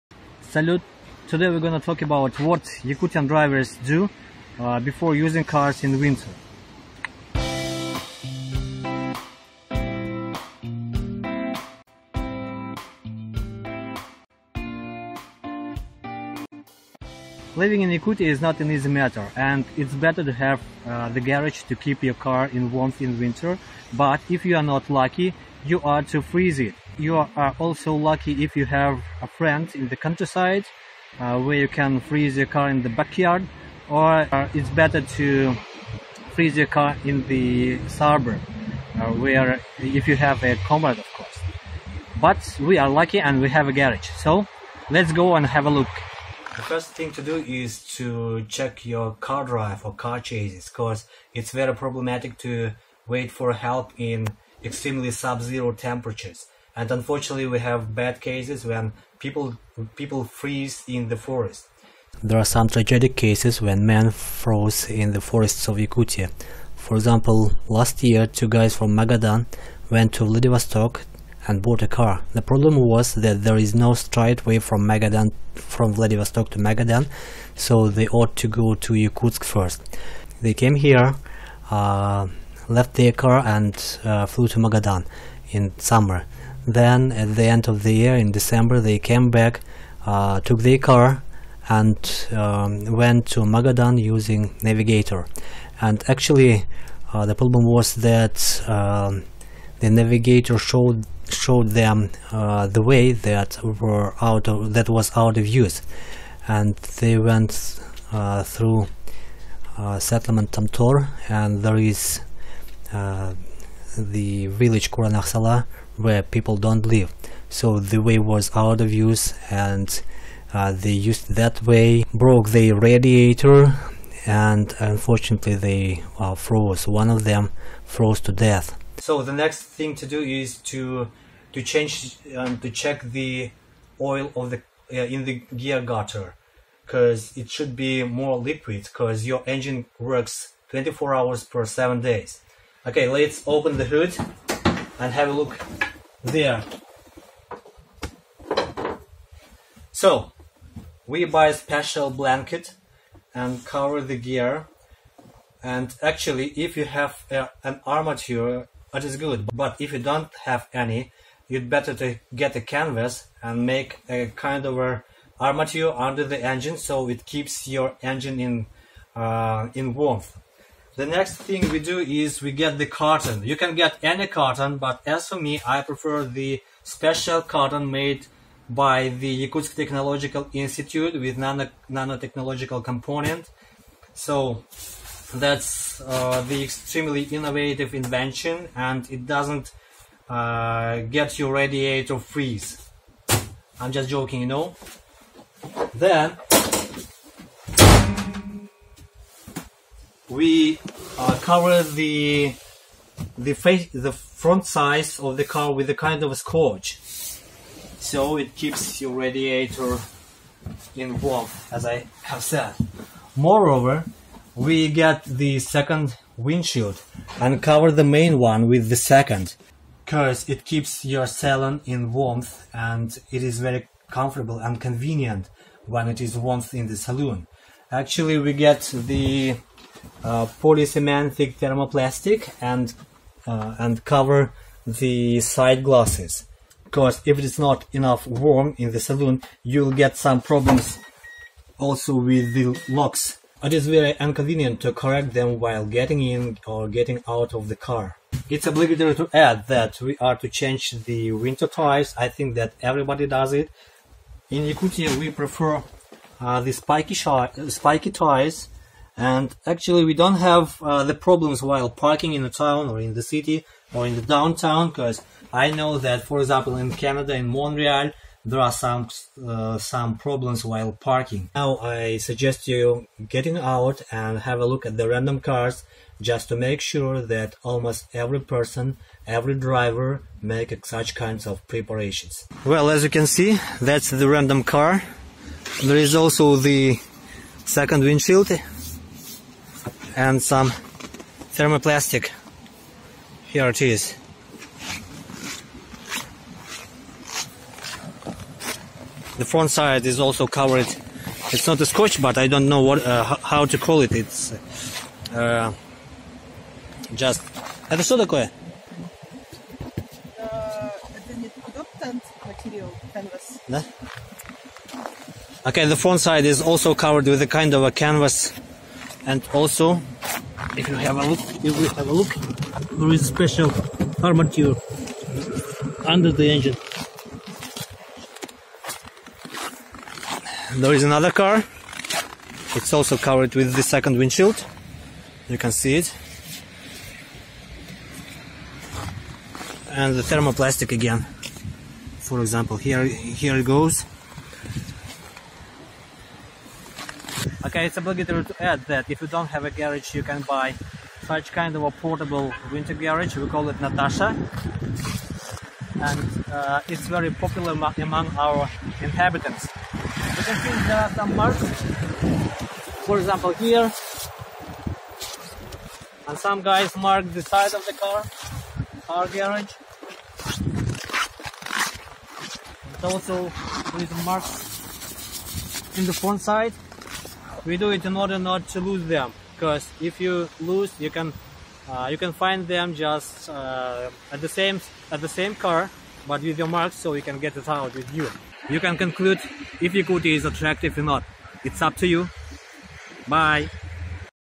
Attractive Yakutia. Part 2. Car season preparations
The second short film about our ordinary life in Yakutia. Here you are getting acquainted with what kind of preparations our drivers do to use a car in winter.
Don't judge me too much as I have edited the film with an axe.